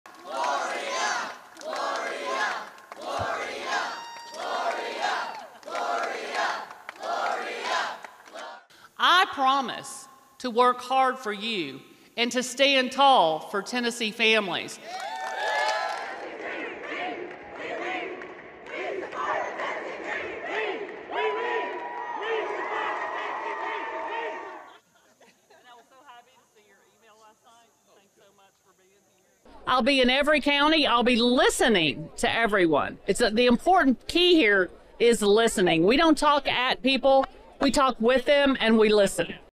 Rep. Gloria Johnson of 'Tennessee Three' officially launches 2024 Senate campaign
Tennessee state Rep. Gloria Johnson has formally announced her campaign for U.S. Senate in 2024. The Democrat shot to fame as a member of the so-called “Tennessee Three" and narrowly survived a Republican-led expulsion effort in the state Legislature.